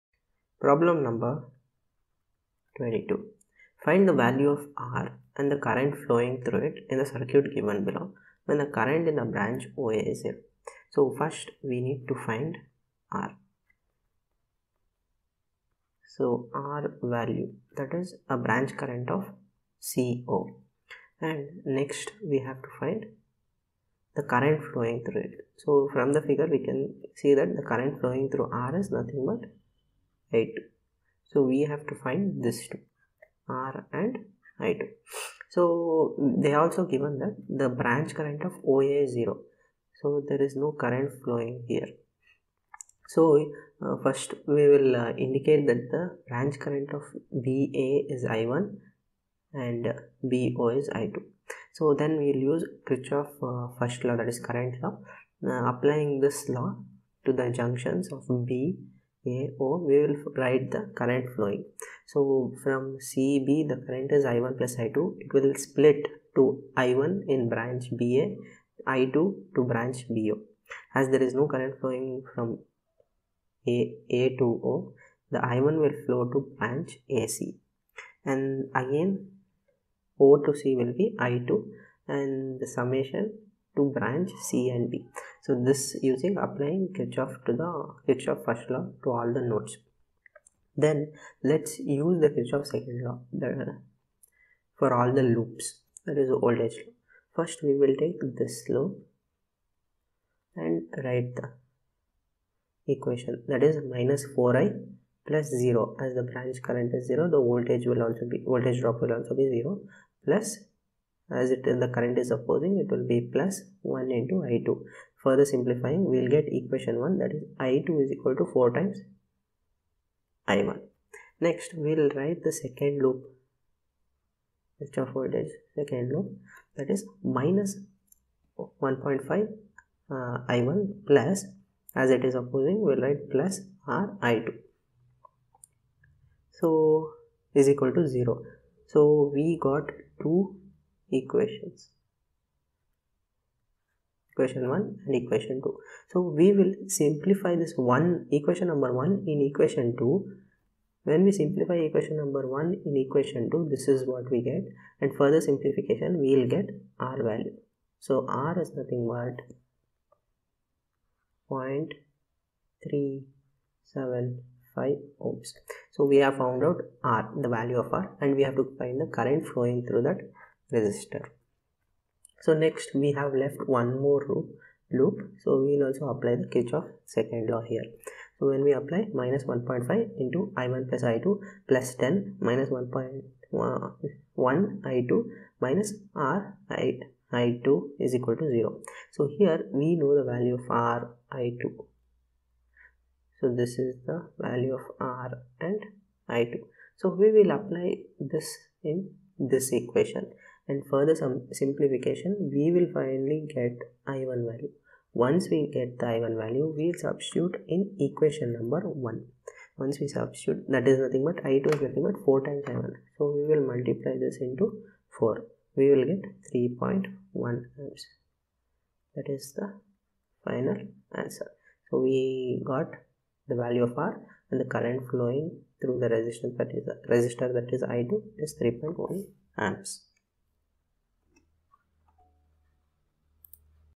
Find the value of Resistance and Current | Problem 1.22 | Rough Book
Problem 1.22: Find the value of R and the current flowing through it in the circuit given below, when the current in the branch OA is Zero.

Rough Book - A Classical Education For The Future!

Rough Book